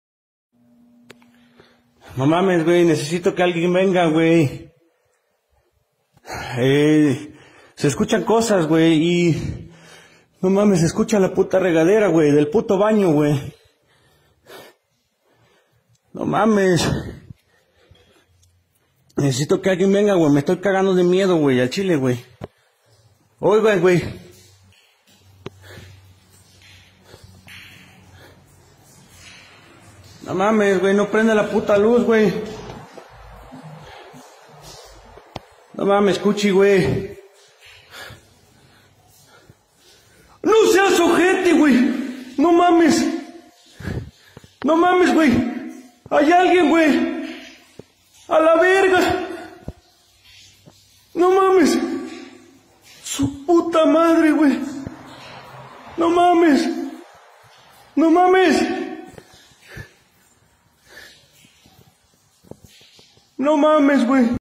Aparición de un espectro en el baño
Crees en los fantasmas, mira en el video y deja tu opinión.